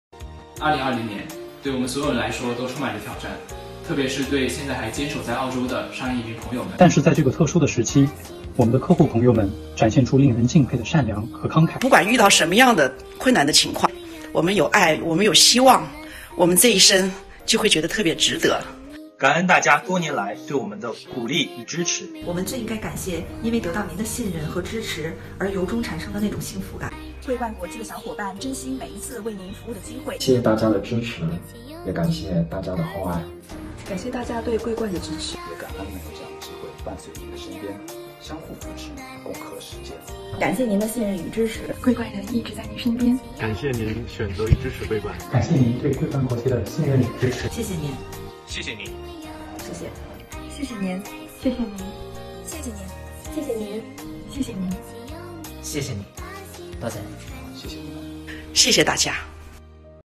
疫情之下，想说一声谢谢您
2020年的疫情，让坚守在澳洲的商业移民朋友们充满挑战。但与此同时，更多的客户朋友们与桂冠国际一起携手，传递爱心，互帮互助。桂冠国际希望能在这特殊时期，为您提供力所能及的帮助。

客户的一封感谢信，字真意切。您为之所感谢的，正是我们应该做的。您的感谢，让我们倍感温暖，湿润眼眶，这是我们最珍贵的礼物。

桂冠的成长离不开每一位小伙伴对客户一点一滴的努力和真挚的付出，我们也很幸运因此收获了更多的祝福和支持，而且倍加珍惜现在的一切。真诚感谢这一路走来，亲人般的客户朋友，对我们的认可与支持。桂冠国际会一直与您相伴！

未来日子，若是美好叫做精彩，若是糟糕也叫做经历，让我们共同携手，共渡难关！